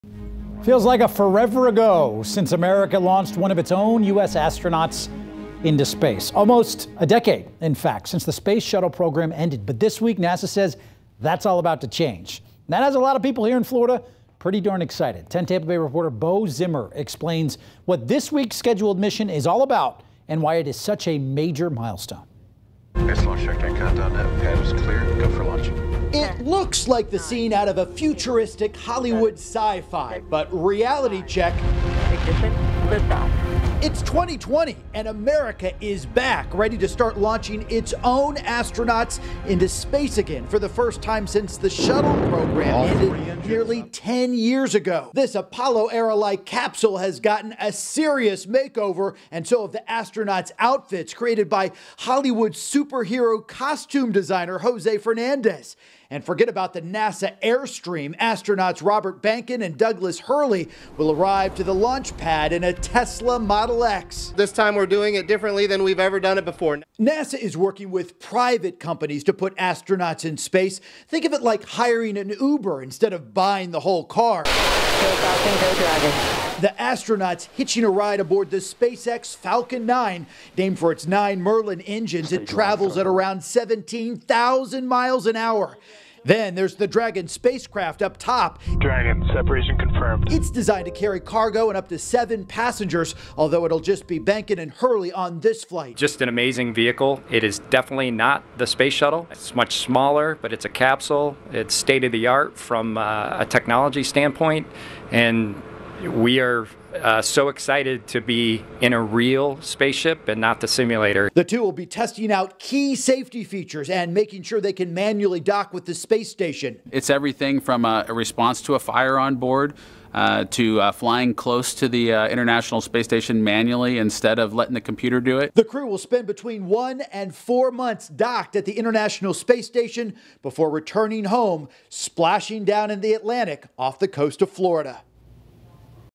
Florida gets excited for return to launching astronauts into space from American soil
The U.S. has relied on other countries to launch U.S. astronauts since the Space Shuttle retired nearly 10 years ago.